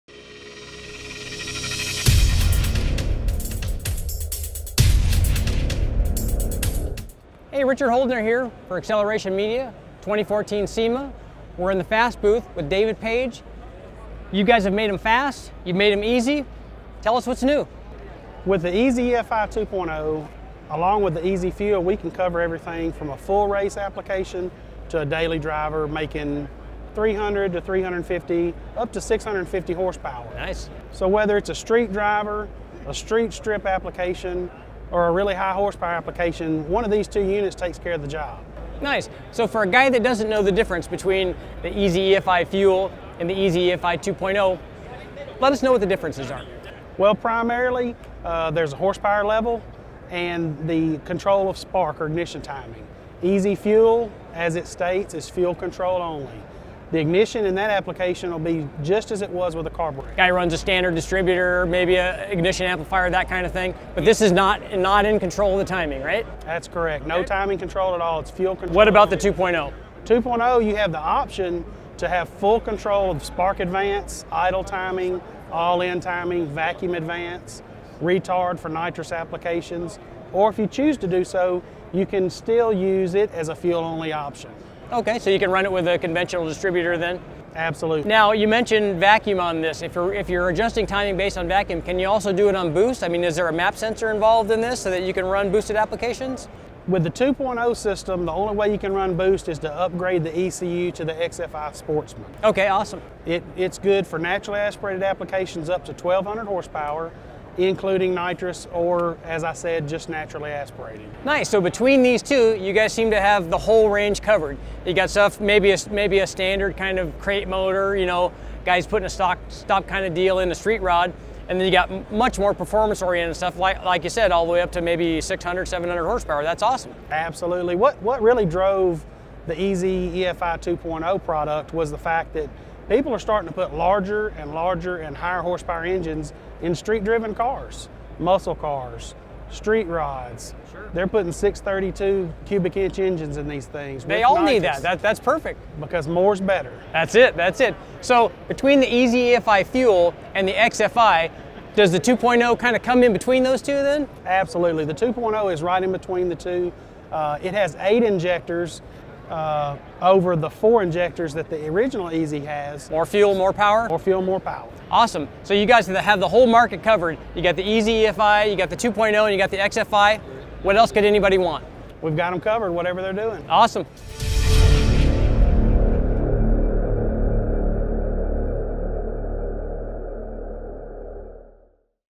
2014 SEMA Show | FAST EZ EFI® Fuel and EZ EFI 2.0® Fuel + Ignition
Showcased at the 2014 SEMA Show, FAST's David Page explains the new developments and benefits of the all new EZ EFI® Fuel and EZ EFI 2.0® Fuel + Ignition systems.